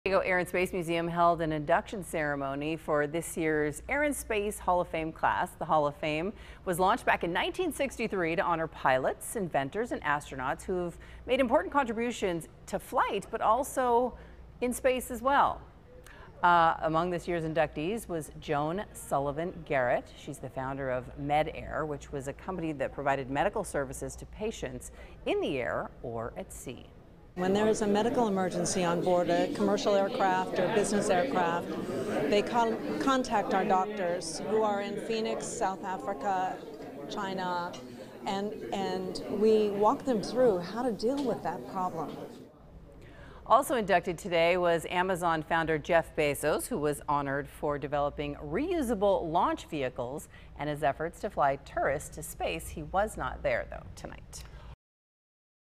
2019 International Air & Space Hall Of Fame
The San Diego Air & Space Museum held an induction ceremony Saturday for its Air & Space Hall of Fame 2019 class.